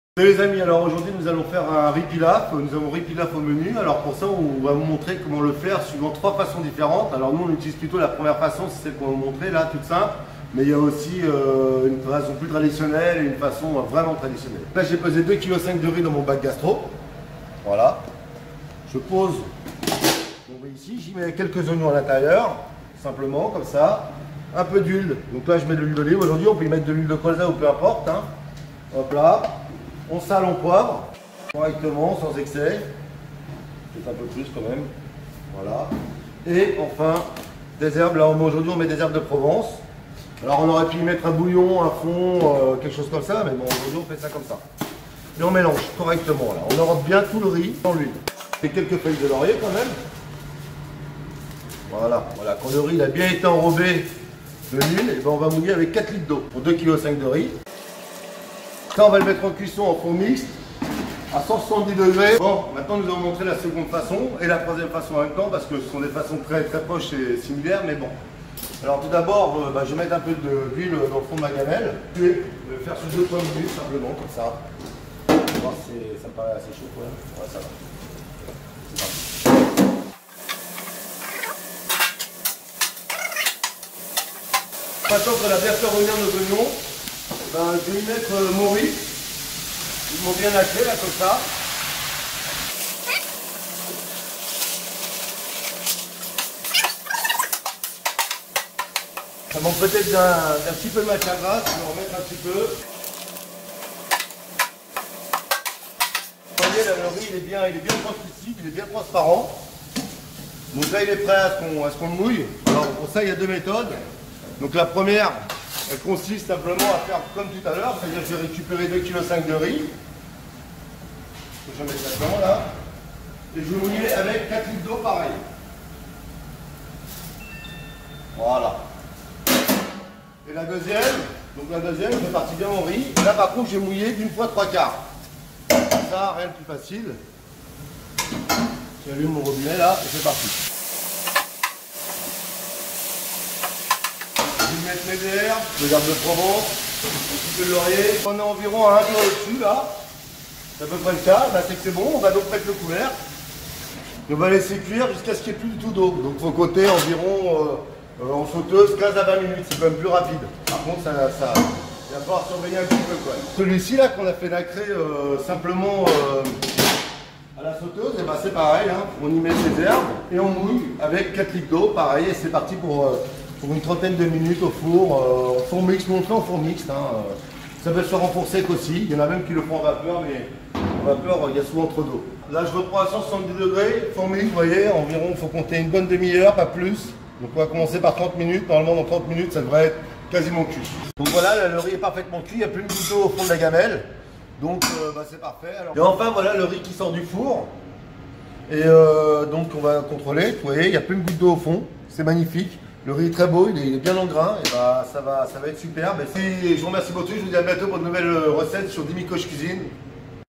Riz pilaf 3 méthodes pour cuisine collective
Riz pilaf ou pilaw Pour 200 à 220 personnes environs
15kg de riz
1 fois 3/4 d'eau bouillante c'est mieux ou de bouillon de fond par litre de riz soit environ 26 litres.
5 kg d oignons
Facultatif des herbes du laurier etc du curry du riz d or ou autre
Salé poivré
Beurre en fin de cuisson mais tout dépend de se que le riz accompagne.